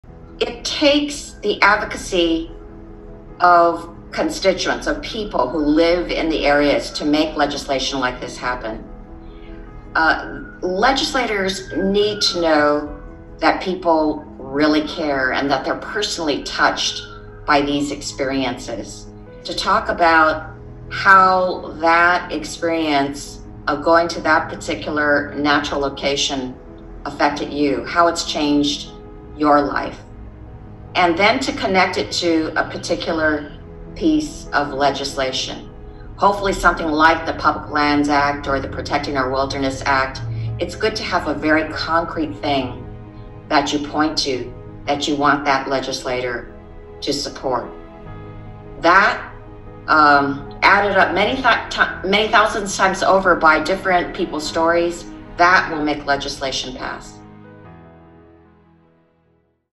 LO Public Lands Shorts with Representative Judy Chu
Representative Judy Chu describes how sharing personal experiences and making a connection to public lands is part of advocacy.

The PUBLIC Lands Act would increase protections for over 1 million acres of federal public lands throughout Northwest California, the Central Coast, and Los Angeles, including nearly 600,000 acres of new wilderness, more than 583 miles of new wild and scenic rivers, and the expansion of an existing national monument by more than 100,000 acres.

This legislation would protect California’s iconic landscapes, provide critical access to open space for communities, support California’s outdoor recreation economy, and fight the climate crisis. Guided by science and input from local communities, the bill also advances the nation’s and California’s goals of conserving 30% of our lands and waters by 2030.

(source: padilla.senate.gov)